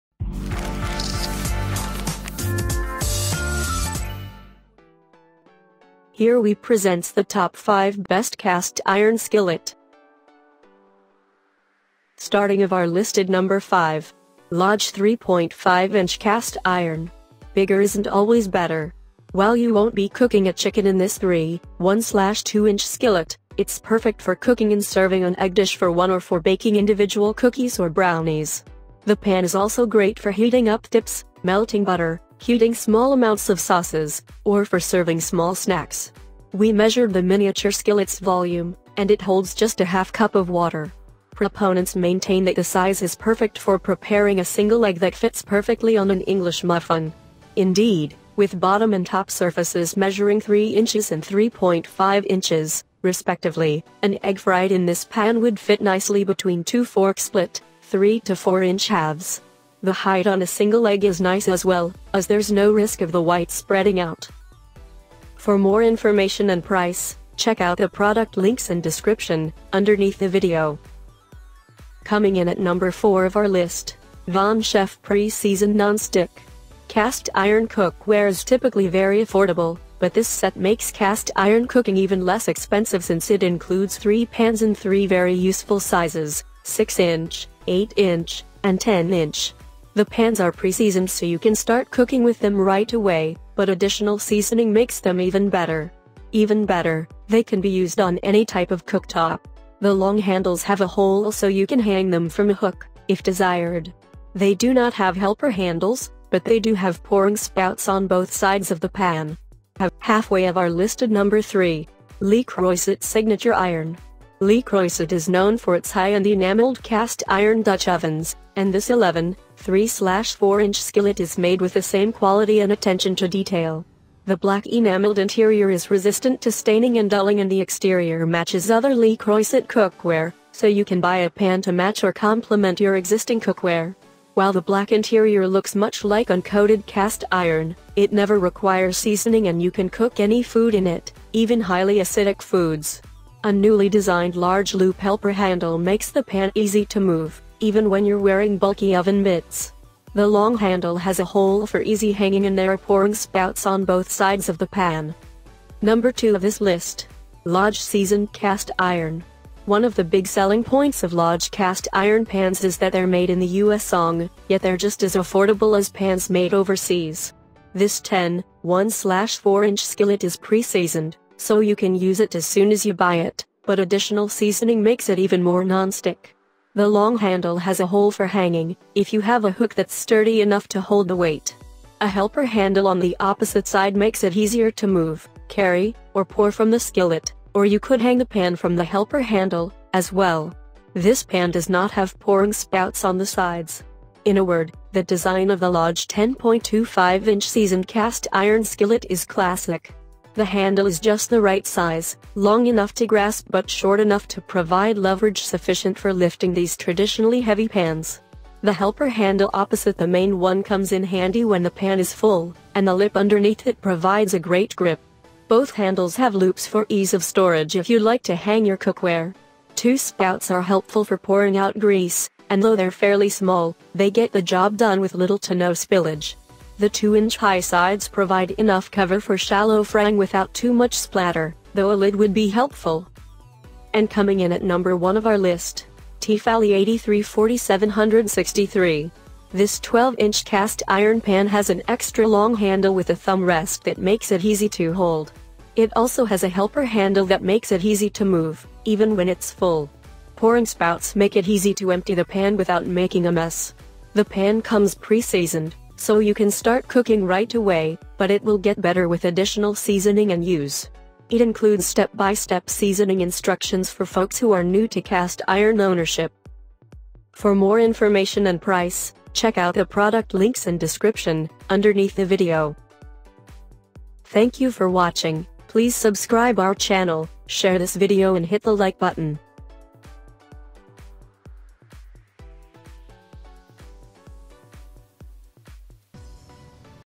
Top 5 Best Cast Iron Skillet 2023 (Buying Guide)
Here is the Updated Prices of the Best Cast Iron Skillet in 2023!

Things that we mentioned in this video:
1. T fal E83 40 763 - B00IWSQCI6
2. Lodge Seasoned Cast Iron - B071FPMVJP
3. VonShef Pre Seasoned Non Stick - B01M7OVTPK
4. Le Creuset Signature Iron - B00B4UOTF2
5. Lodge 5 Inch Cast Iron - B000LXA9YI